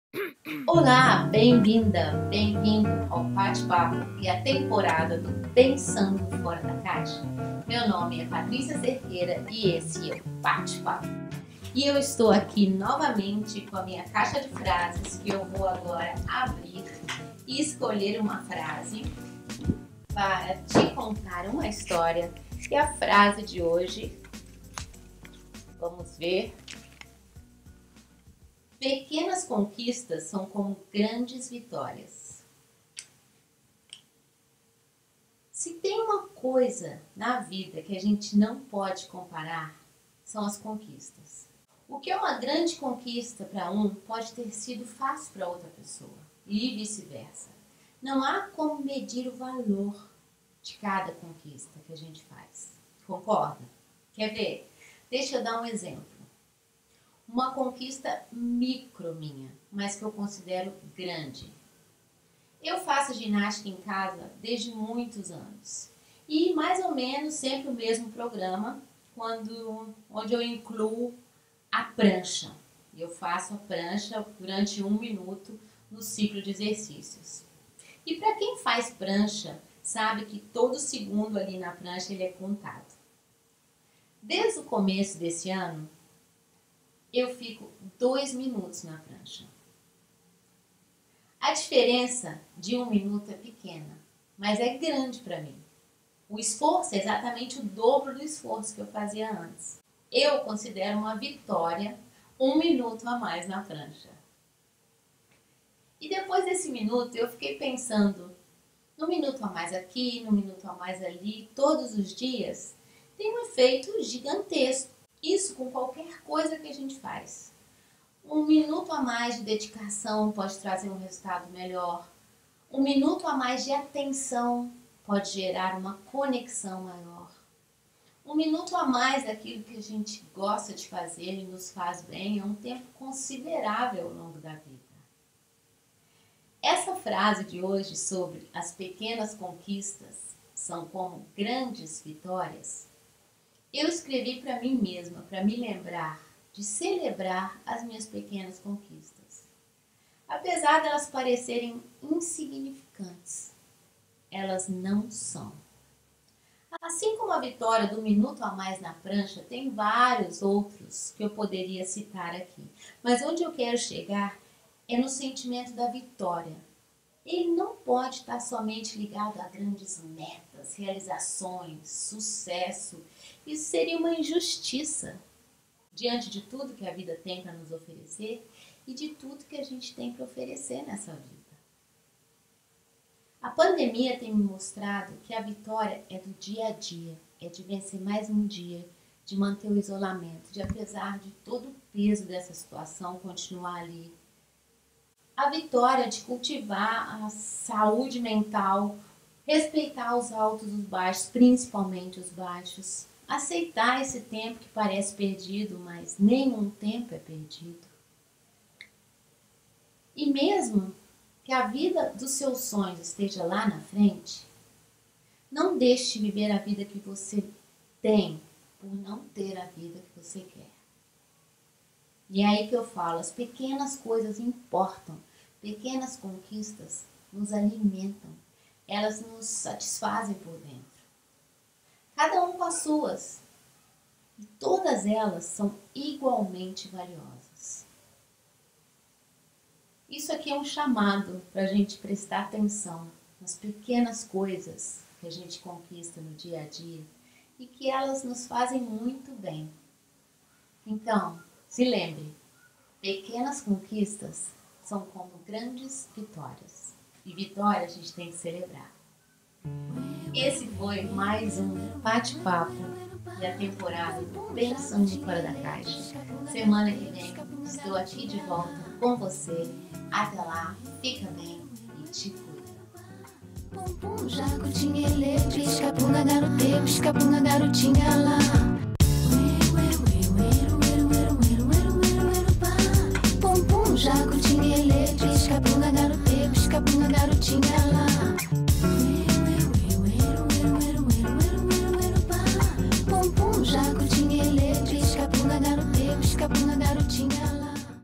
#031 - A Vitória da Conquista
Conquistas não se medem.
Cada uma tem o seu valor.
Nesse episódio eu falo da importância que as pequenas conquistas têm na nossa vida.
Não é preciso conquistar grandes coisas para sentir o sabor da vitória.

PODCAST ON:
Spotify / Stitcher / Apple Podcasts / Google Podcasts / Deezer / TuneIn